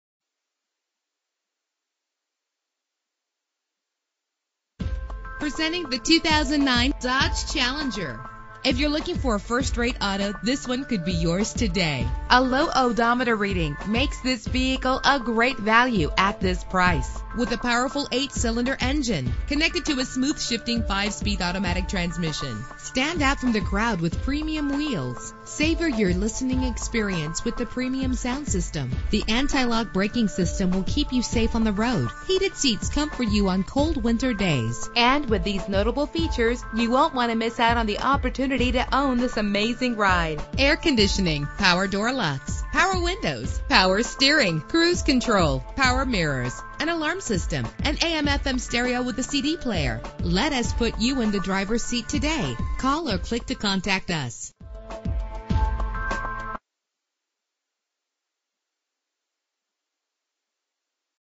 2009 Dodge Challenger - Amarillo TX
Year: 2009
Make: Dodge
Model: Challenger
Trim: SRT8
Engine: 6.1 liter 8 cylinder 16 valve
Transmission: 5-Speed Automatic
Color: Toreador Red
Mileage: 18011
Address:
7800 I-40 West